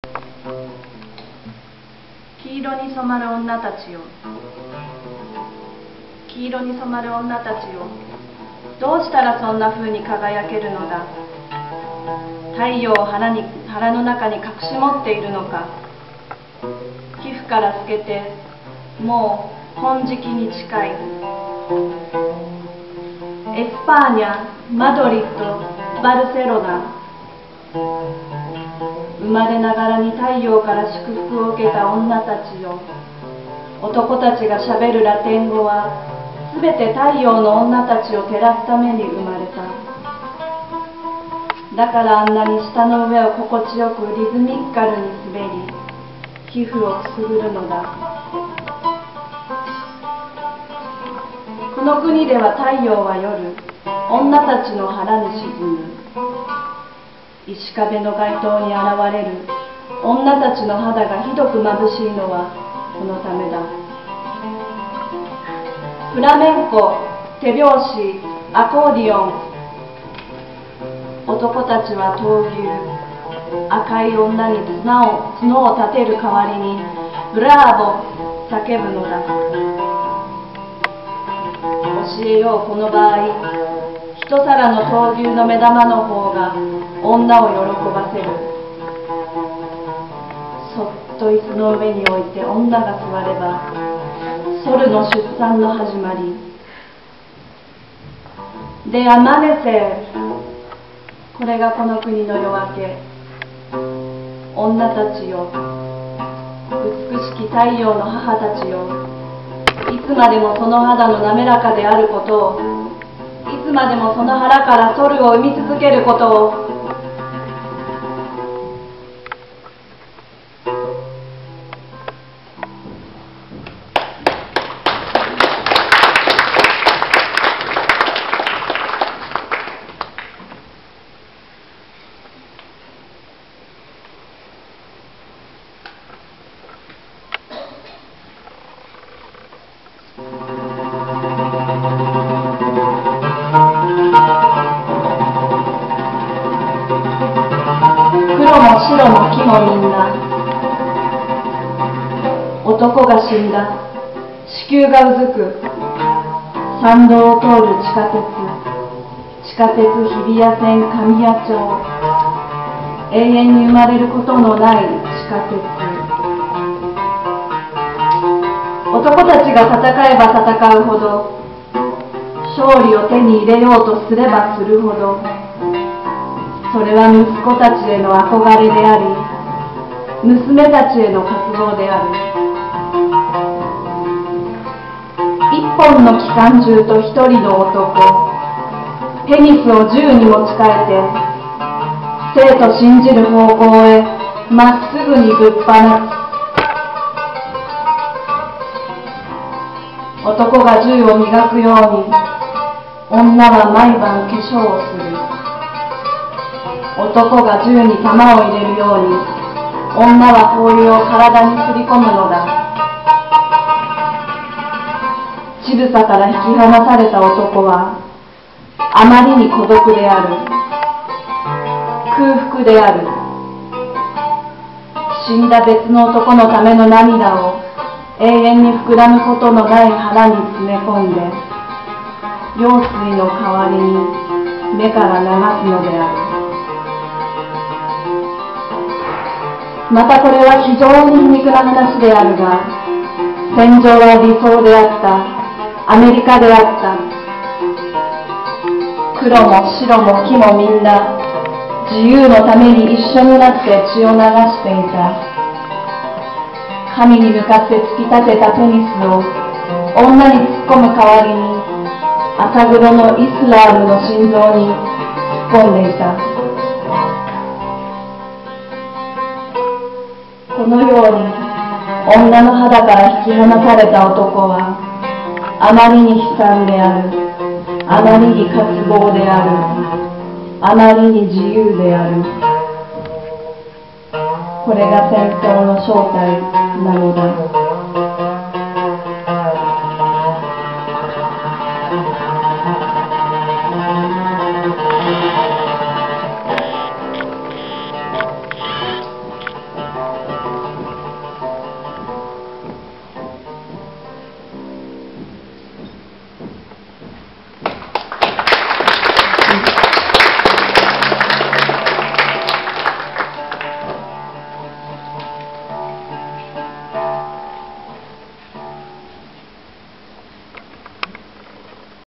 Lecture de poèmes du Japon... Japanese poetry reading... (part 1/3)
Lecture de poèmes du Japon par Saori Fukasawa,
avec le danseur,  le poète du corps Kazuma Glen Motomura
accompagnée à l'Oud par Abdelhamid Cherragui.

Japanese poetry reading by Saori Fukasawa
with the dancer Kazuma Glen Motomura, the bodypoet.
accompanied with the Oud music by Abdelhamid Cherragui.

30/03/2011.
Grenoble, France.